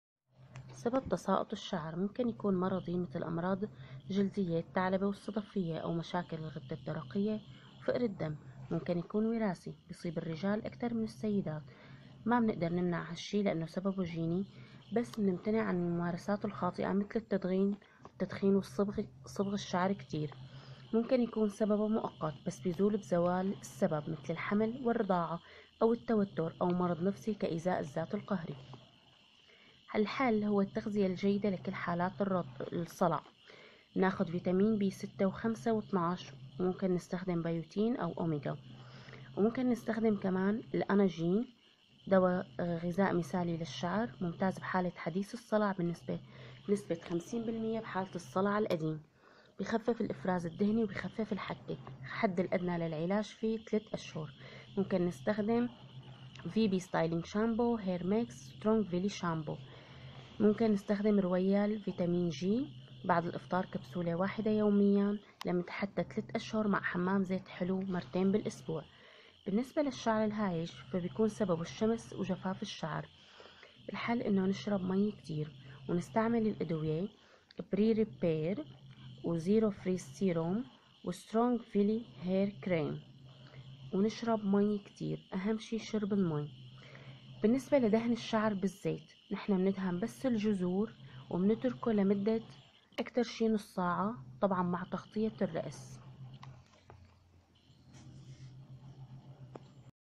تساقط الشعر و أسبابه و علاجه دوائيا و طبيعيا
الزيت يسبب جفاف الشعر و أغلب الناس لا تعرف هذه المعلومة لهذا ننصح بوضع الزيت على الجذور فقط و مدة لا تزيد عن نصف ساعة
تغطيته بقماس ساخن حتى يفتح مسام الرأس
شكراً لدعمكم